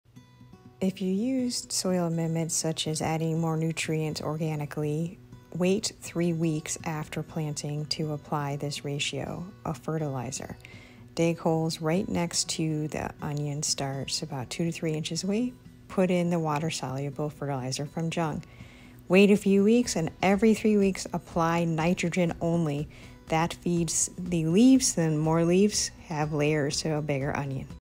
How To Fertilize Onion 🧅 Plants
At planting of onion starts I used new rich raised bed soil and worm castings. I waited 3 weeks till used the Jung Fertilizer ration NPK15-30-15. This is a tutorial as to how apply the fertilizer and when to switch to just a nitrogen source.